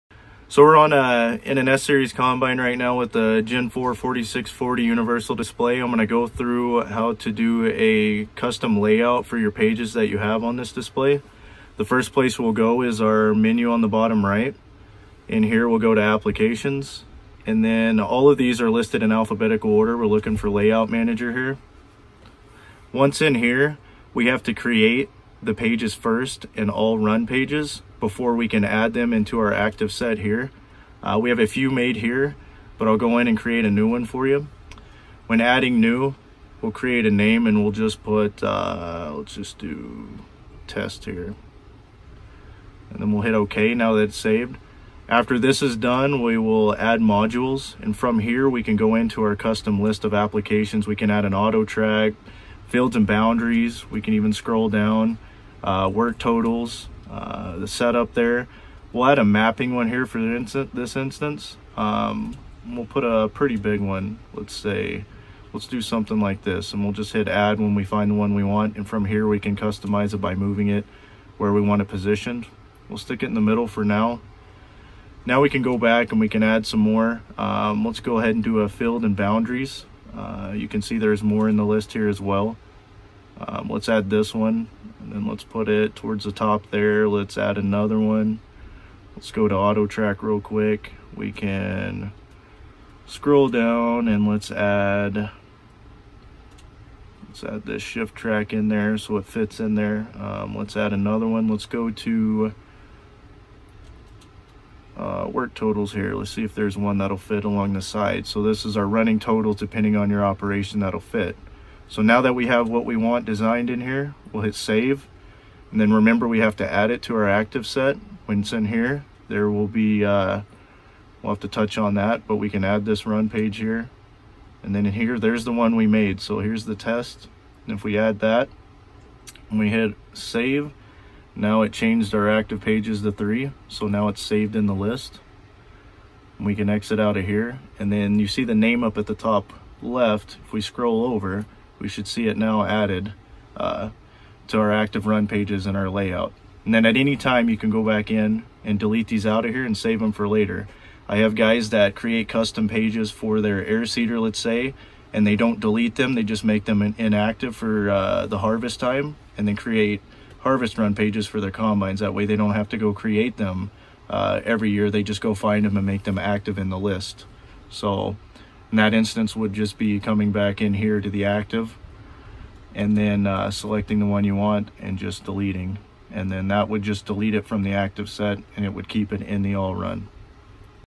Creating a Custom Run Page Layout in a John Deere Gen 4 Display
It’s easy to customize your Run Pages in a John Deere Gen 4 Display. Leading Edge Equipment’s Kevin O’Dell shows you how to create the layout that works for you, along with tips on how to manage your Active Run Pages throughout the year.